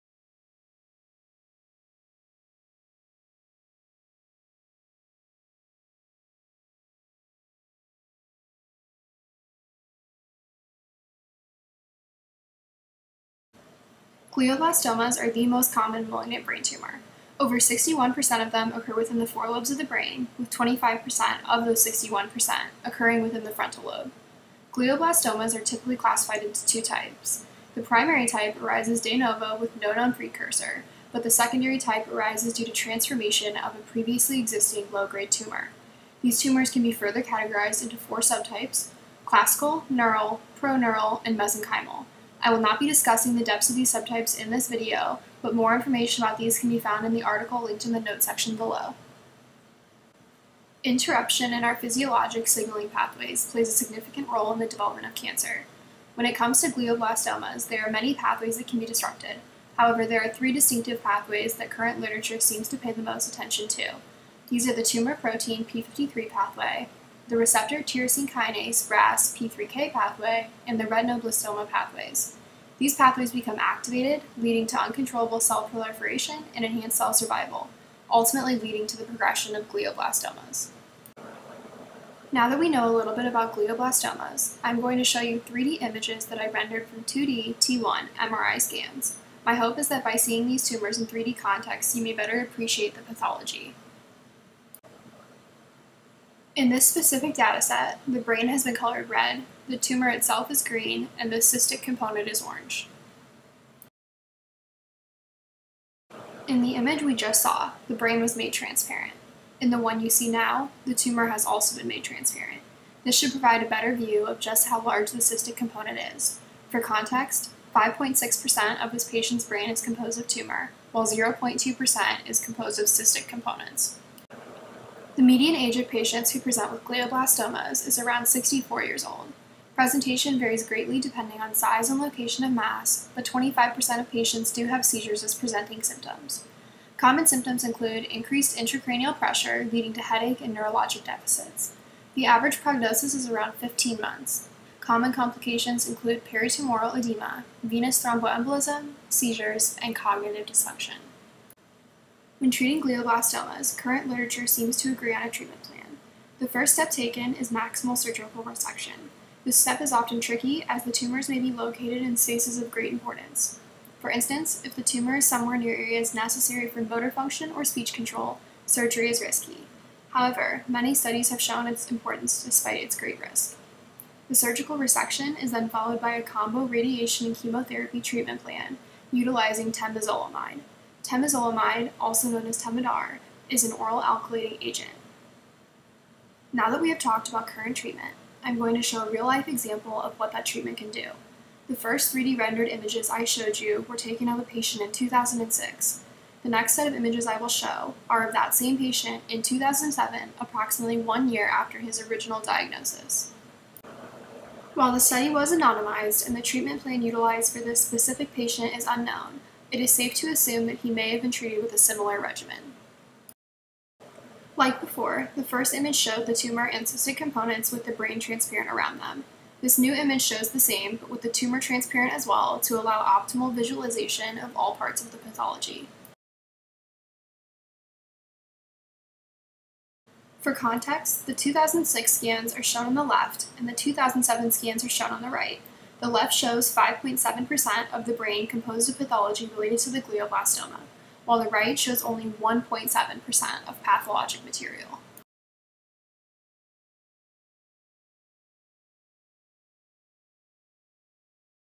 3D Visualization of Glioblastomas
This video introduces the pathology of glioblastoma, and common interventions. We also explore a longitudinal study showing the treatment progression of an anonymized patient. The source data were derived from the NIH Cancer Imaging Archives and used to render 3D visualizations from sectional imaging studies such as T2 weighted MRI.